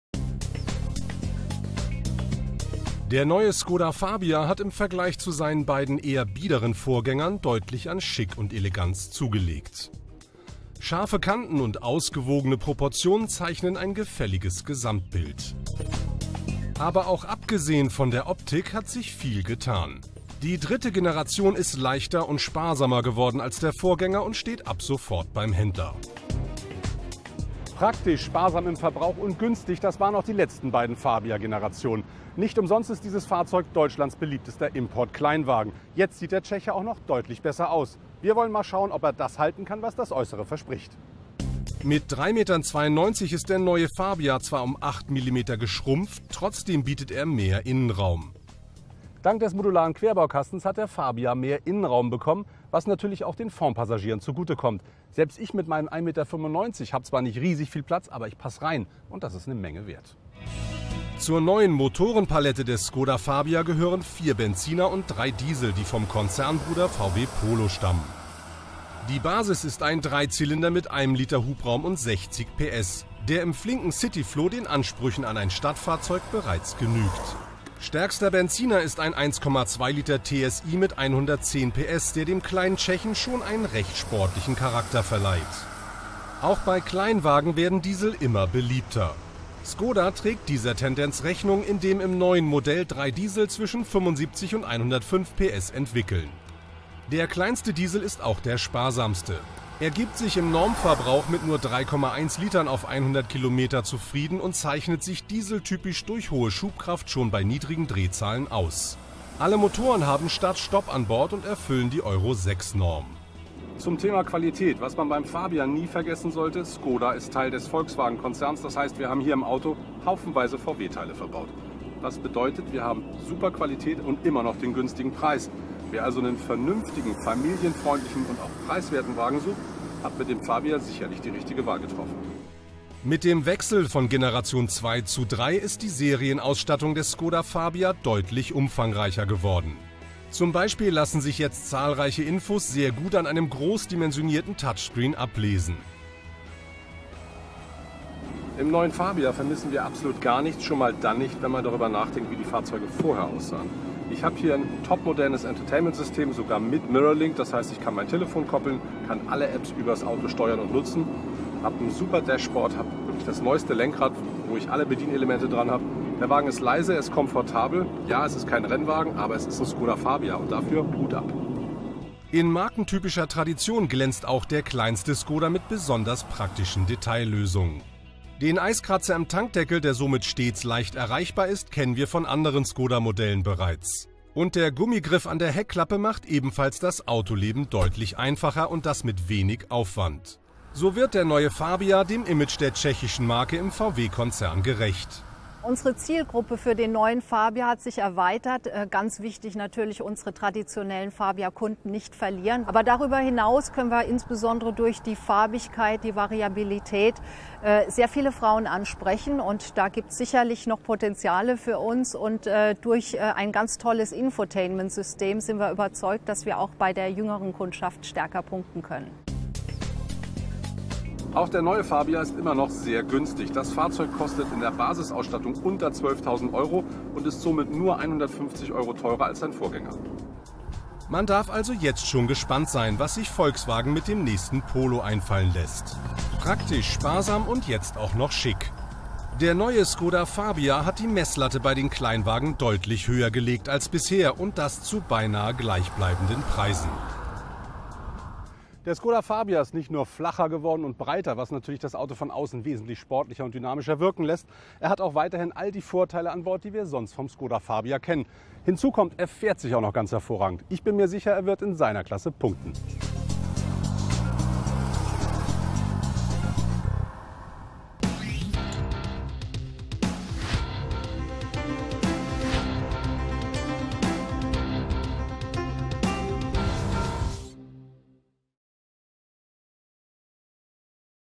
Skoda Fabia 3. Generation
Bei der Entwicklung boten sich viele Gelegenheiten, neueste Trends zu integrieren. Fahrer werden nicht nur die Laufruhe zu schätzen wissen, sondern auch mehr